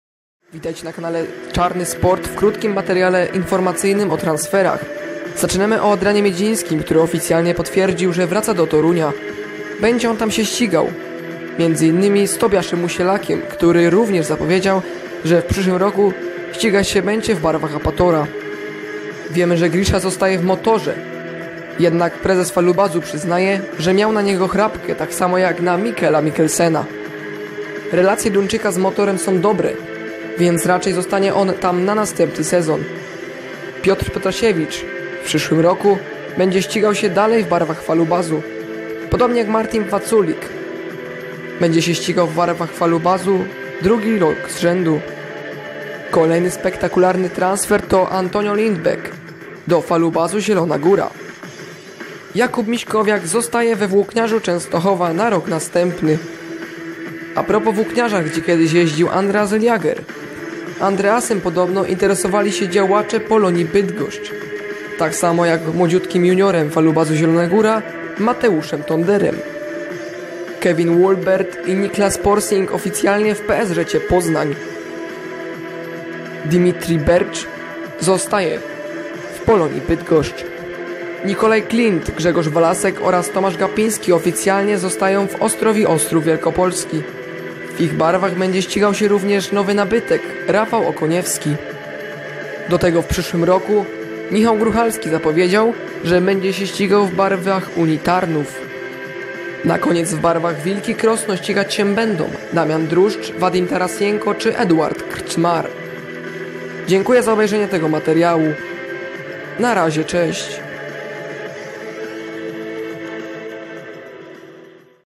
TRANSFER NEWS
Podsumowanie transferów po połowie okienka ;)

Taki krótki odcinek informacyjny